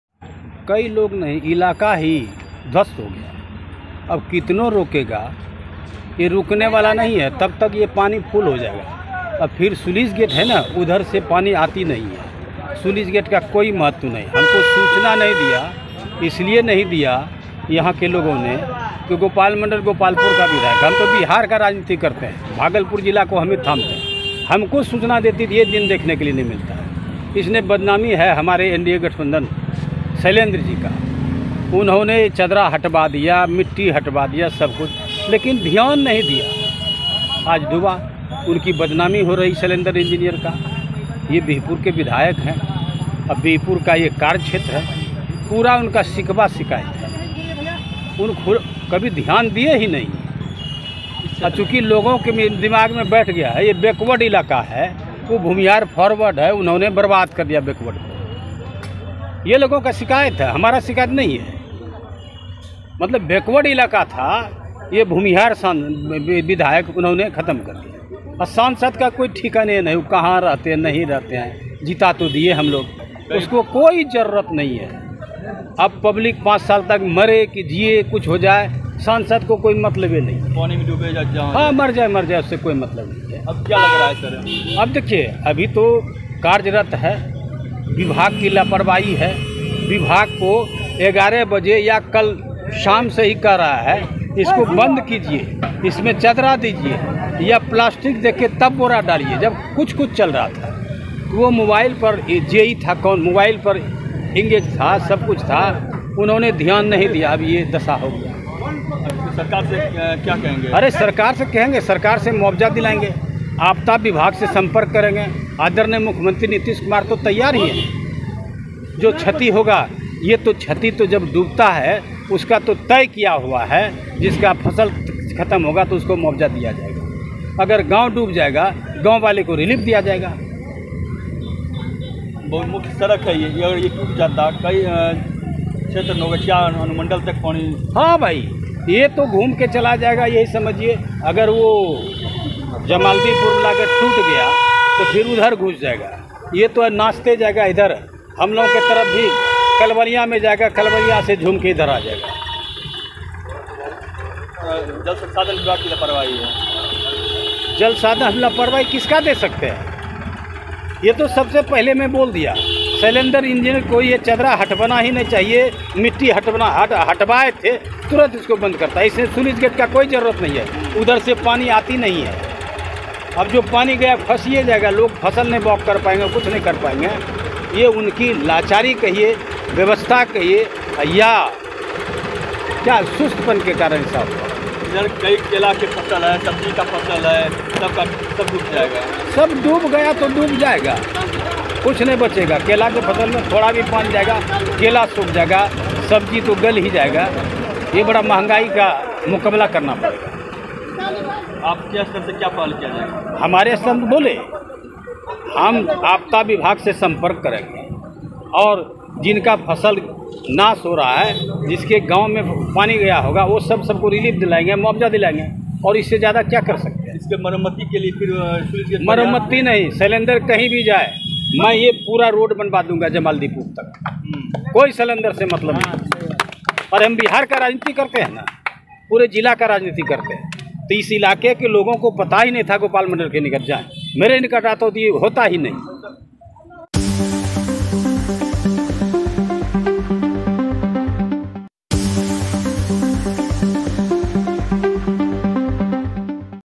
JDU विधायक गोपाल मंडल ने Bhagalpur में बाढ़ को लेकर सरकार को ठहराया जिम्मेदार,MP-MLA पर निकाली भड़ास
JDU विधायक गोपाल मंडल ने Bhagalpur में बाढ़ को लेकर सरकार को ठहराया जिम्मेदार, MP और MLA पर निकाली भड़ास